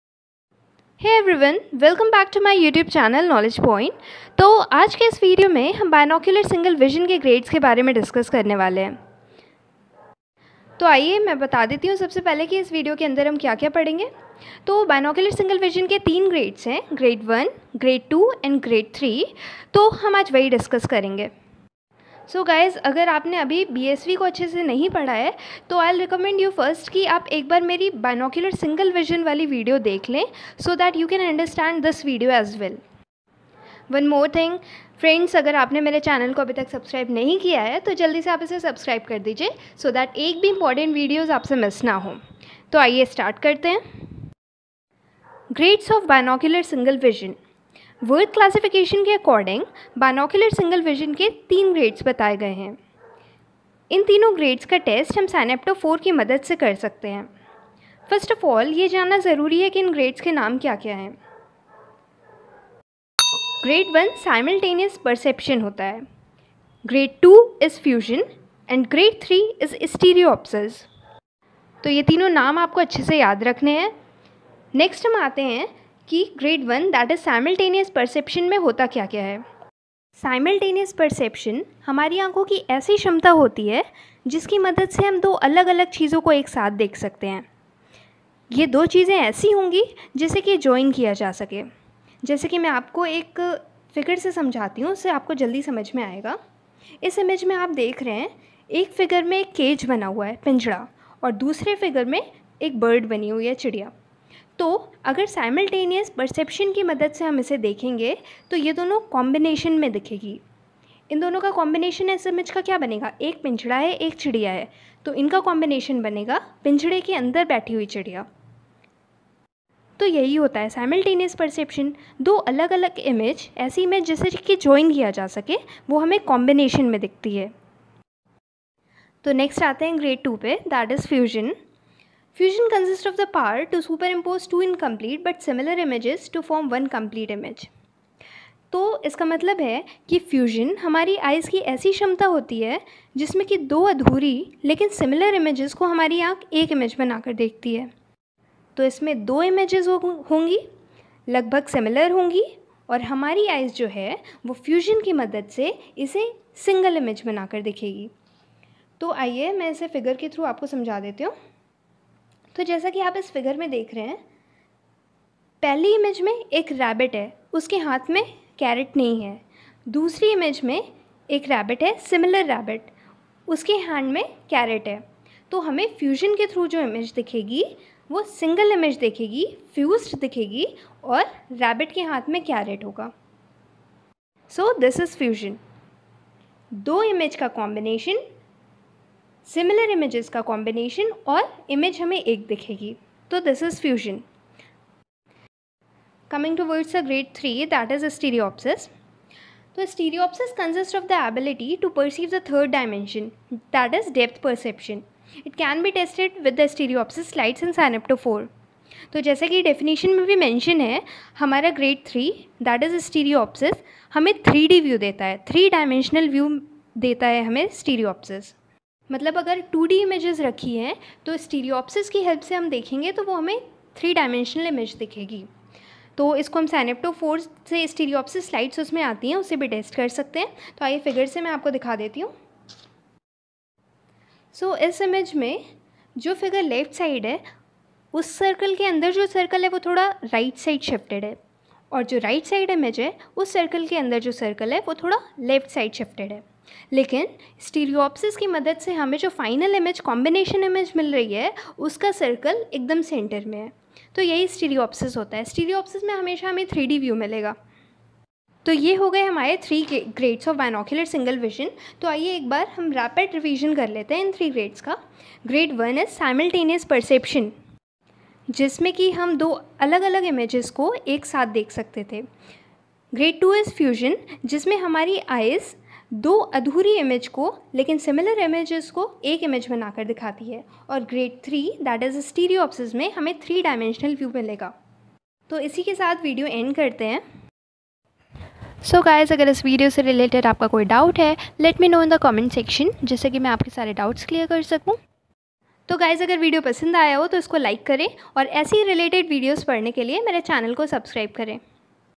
Grades of Binocular single vision | simultaneous perception | Fusion | Stereopsis
Hey everyone, in this video you will get to know about the 3 grades of binocular single vision. All the heads under this video is explained in easiest possible manner with proper diagrams.